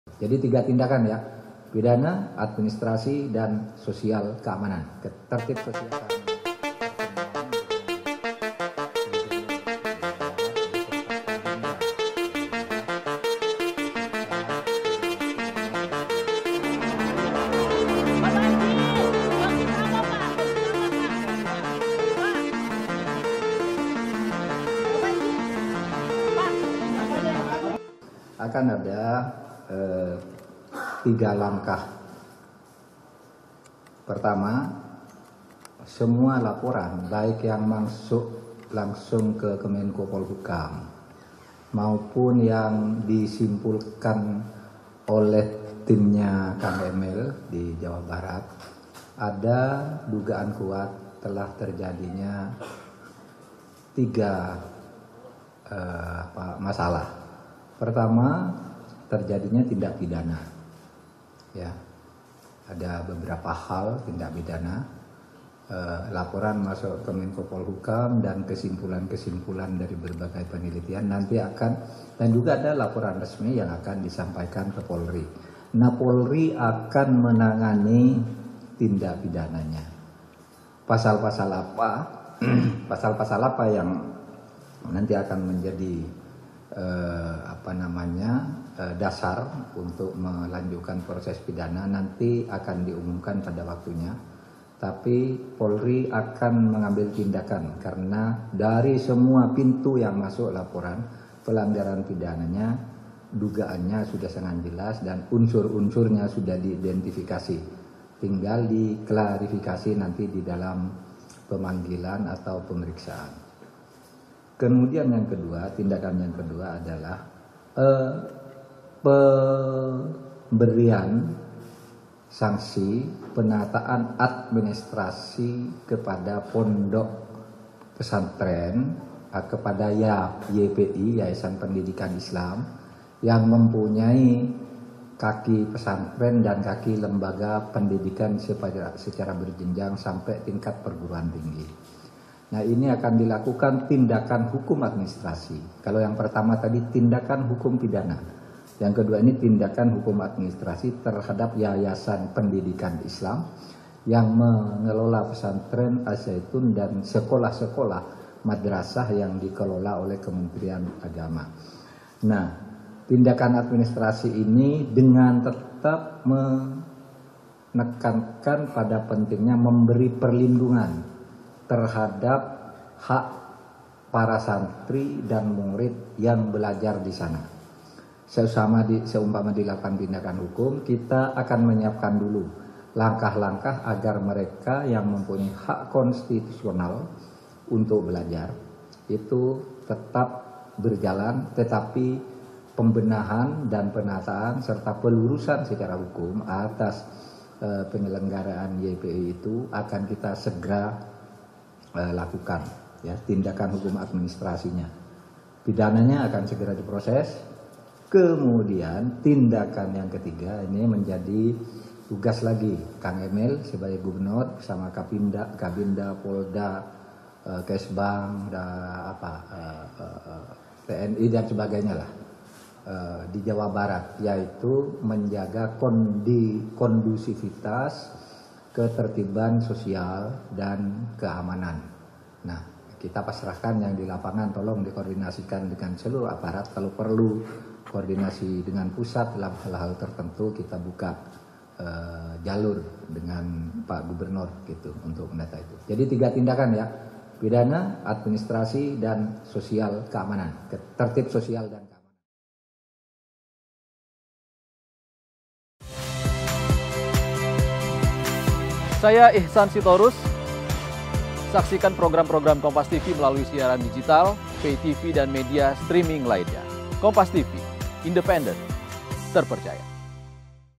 Mahfud MD Bakal Lakukan 3 Langkah Tindak Lanjuti Polemik Al Zaytun
KOMPAS.TV- Mahfud MD Bakal Lakukan 3 Langkah Tindak Lanjuti Polemik Al Zaytun